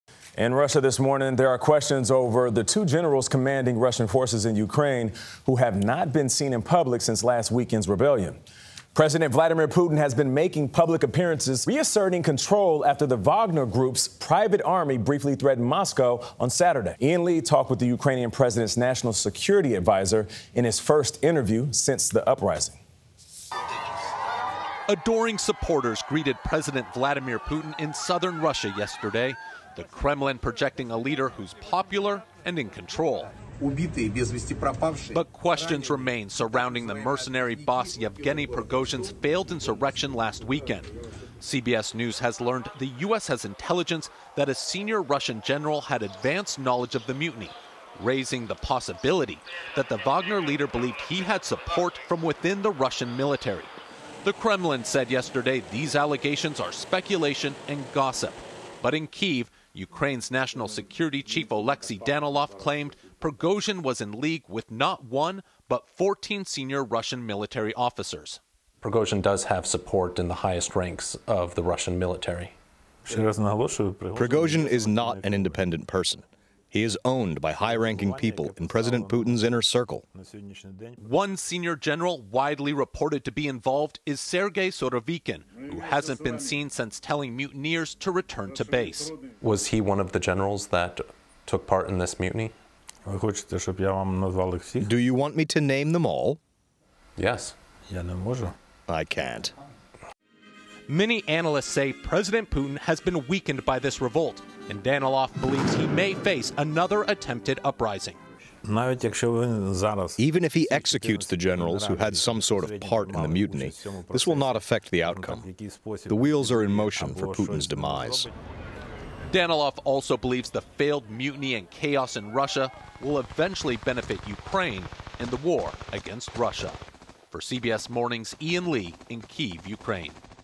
Ukraine top security official says Vladimir Putin's own generals are working against him
Ian Lee sits down with Ukraine's national security adviser, Oleksiy Danilov, who told him they believe Wagner Group chief Yevgeny Prigozhin was not working alone when he tried to bring down Russia's military establishment. Danilov says Prigozhin is controlled by high-ranking officials in Putin's inner circle who want to bring about a change in Russia's leadership — and as a result, he believes Putin's days are numbered.